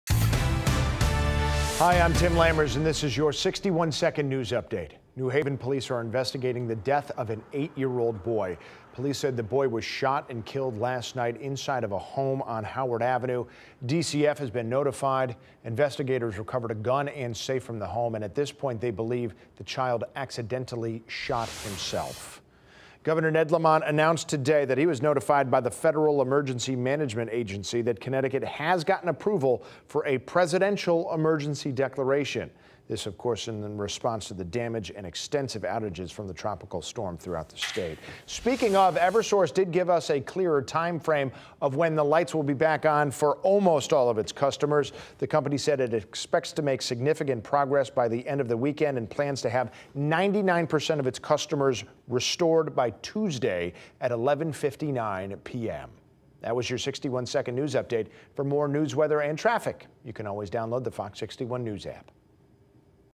61 Second News: August 7
8 year old dies from a believed accidental shooting inside a New Haven home; Isaias clean up continues